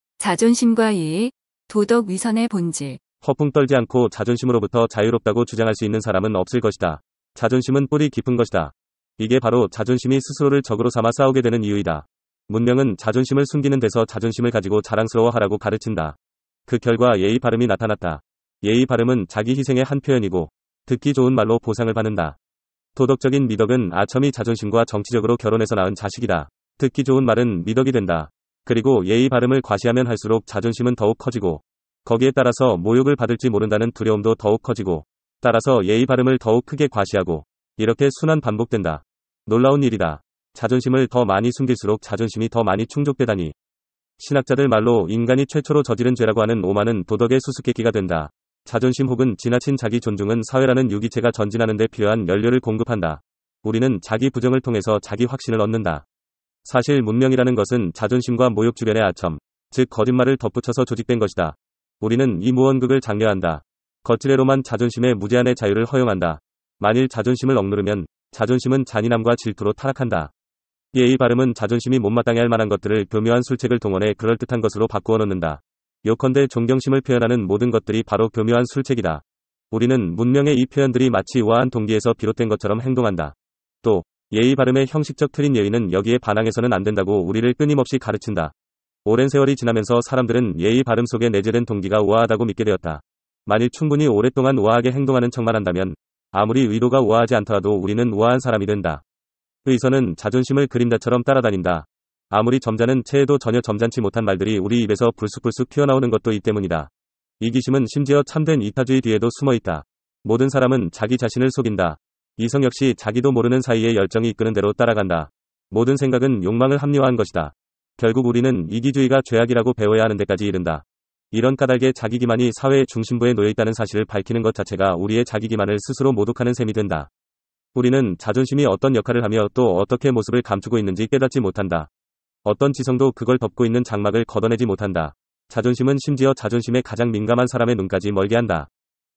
자존심과 예의, 도덕 위선의 본질
허풍 떨지 않고 자존심으로부터 자유롭다고 주장할 수 있는 사람은 없을 것이다.  자존심은 뿌리 깊은 것이다.  이게 바로 자존심이 스스로를 적으로 삼아 싸우게 되는 이유이다.  문명은 자존심을 숨기는 데서 자존심을 가지고 자랑스러워 하라고 가르친다.  그 결과 ‘예의 바름’이 나타났다.  ‘예의 바름’은 자기 희생의 한 표현이고, 듣기 좋은 말로 보상을 받는다.
 ‘도덕적인 미덕은 아첨이 자존심과 정치적으로 결혼해서 낳은 자식이다.’ 듣기 좋은 말은 미덕이 된다.  그리고 ‘예의 바름’을 과시하면 할수록 자존심은 더욱 커지고, 거기에 따라서 모욕을 받을지 모른다는 두려움도 더욱 커지고, 따라서 ‘예의 바름’을 더욱 크게 과시하고, 이렇게 순환 반복된다.  놀라운 일이다.   자존심을 더 많이 숨길수록 자존심이 더 많이 충족되다니,
신학자들 말로 인간이 최초로 저지른 죄라고 하는 오만은 도덕의 수수께끼가 된다.  자존심 혹은 지나친 자기 존중은 사회라는 유기체가 전진하는데 필요한 연료를 공급한다.  우리는 자기 부정을 통해서 자기 확신을 얻는다.
사실 문명이라는 것은 자존심과 모욕 주변에 아첨, 즉 거짓말을 덧붙여서 조직된 것이다.  우리는 이 무언극을 장려한다.  겉치레로만 자존심에 무제한의 자유를 허용한다. 만일 자존심을 억누르면, 자존심은 잔인함과 질투로 타락한다.
‘예의 바름’은 자존심이 못마땅해 할 만한 것들을 교묘한 술책을 동원해 그럴듯한 것으로 바꾸어놓는다. 요컨대 존경심을 표현하는 모든 것들이 바로 교묘한 술책이다.  우리는 문명의 이 표현들이 마치 우아한 동기에서 비롯된 것처럼 행동한다.  또, ‘예의 바름’의 형식적 틀인 예의는 여기에 반항해서는 안 된다고 우리를 끊임없이 가르친다.  오랜 세월이 지나면서 사람들은 ‘예의 바름’ 속에 내재된 동기가 우아하다고 믿게 되었다.  만일 충분히 오랫동안 우아하게 행동하는 척만 한다면, 아무리 의도가 우아하지 않더라도 우리는 우아한 사람이 된다.
위선은 자존심을 그림자처럼 따라다닌다.  아무리 점잖은 체해도 전혀 점잖지 못한 말들이 우리 입에서 불쑥불쑥 튀어나오는 것도 이 때문이다.  이기심은 심지어 참된 이타주의 뒤에도 숨어 있다.  모든 사람은 자기 자신을 속인다.  이성 역시 자기도 모르는 사이에 열정이 이끄는 대로 따라간다.  모든 생각은 욕망을 합리화한 것이다.  결국 우리는 이기주의가 죄악이라고 배워야 하는 데까지 이른다.  이런 까닭에 자기기만이 사회의 중심부에 놓여 있다는 사실을 밝히는 것 자체가 우리의 자기기만을 스스로 모독하는 셈이 된다.
우리는 자존심이 어떤 역할을 하며 또 어떻게 모습을 감추고 있는지 깨닫지 못한다.  어떤 지성도 그걸 덮고 있는 장막을 걷어내지 못한다.  자존심은 심지어 자존심에 가장 민감한 사람의 눈까지 멀게 한다.